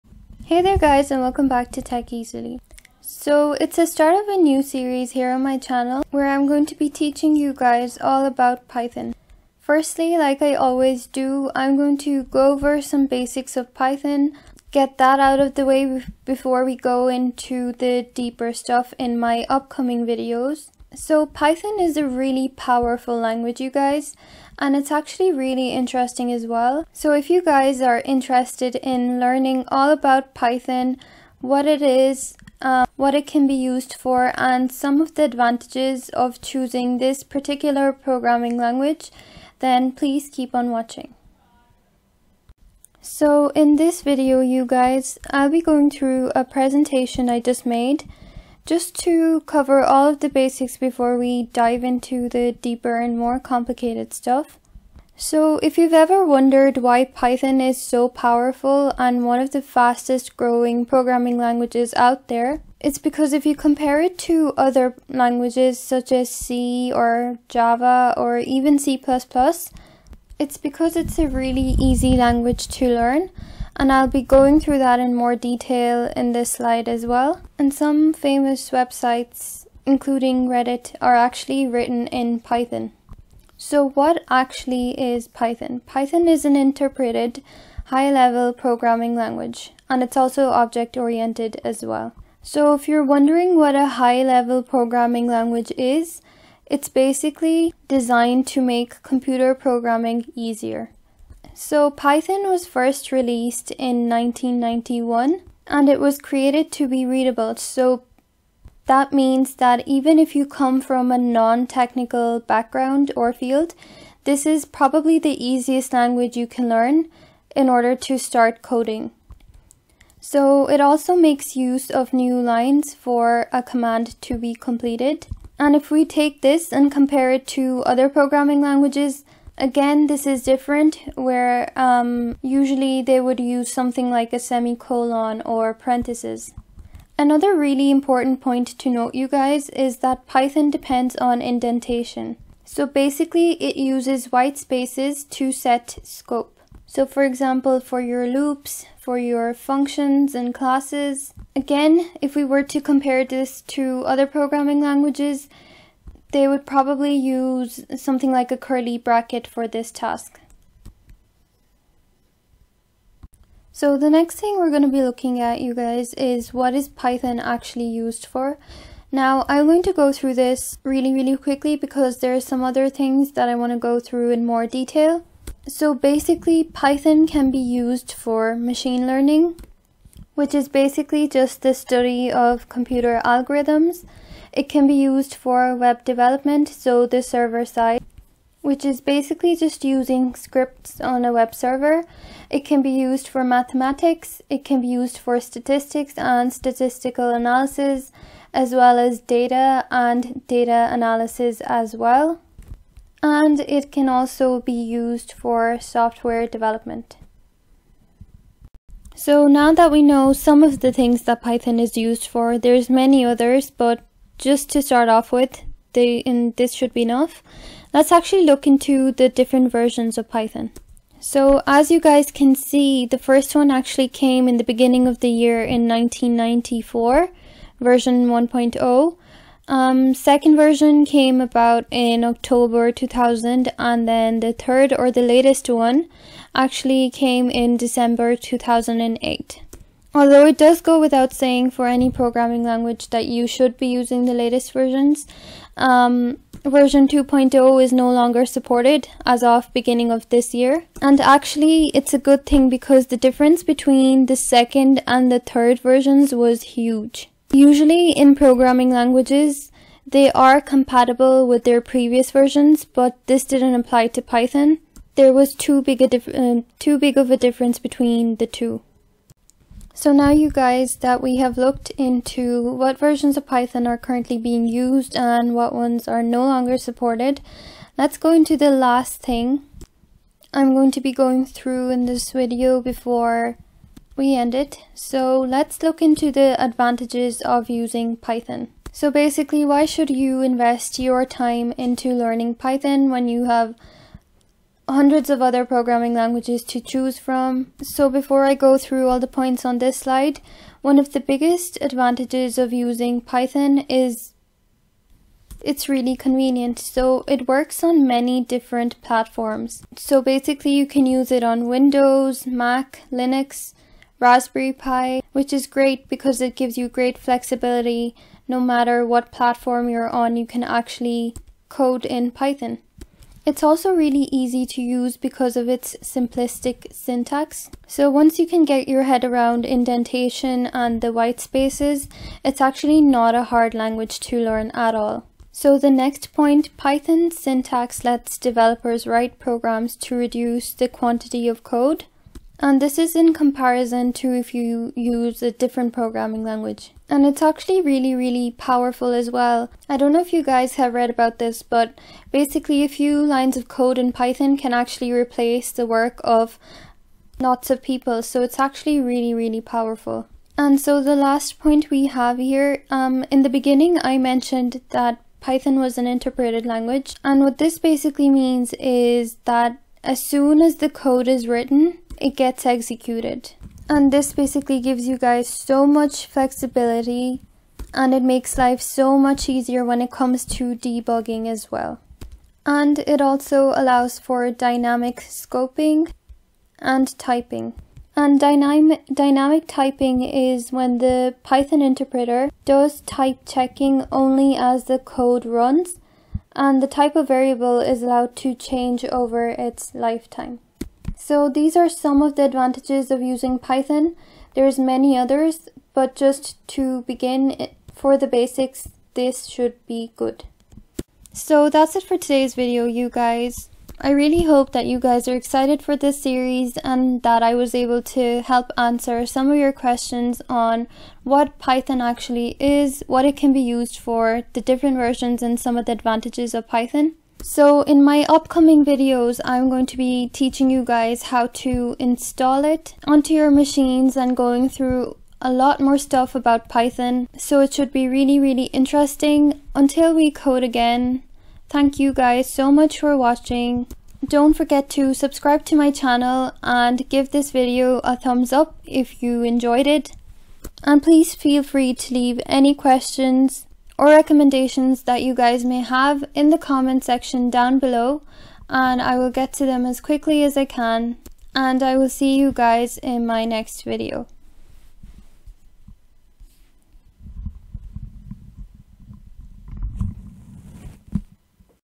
PYTHON tutorial for BEGINNERS | Learn Basics of Python Programming 2024 | WHAT IS PYTHON | part 1
This video contains information about the basics of Python. I have shown this in an easy way and I hope it helps you guys! If you have any suggestions or questions, leave them down below.

PYTHON tutorial for BEGINNERS | Learn Basics of Python Programming 2021 | WHAT IS PYTHON | part 1